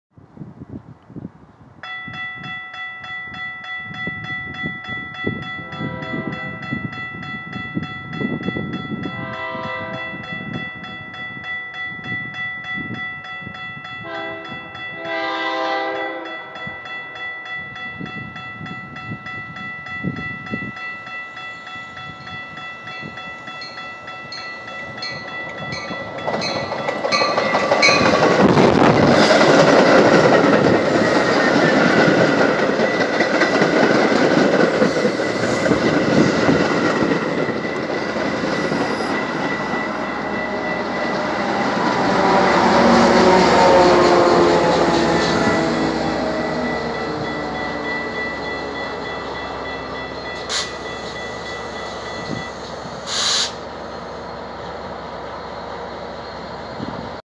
Metra cab-car 8511 east at National St. in Elgin, Illinois with F59PHI 88 on November 16, 2021
Metra cab-car 8511 leads a Chicago bound train into the station stop at National St. in Elgin, Illinois with F59PHI 88 for power on November 16, 2021.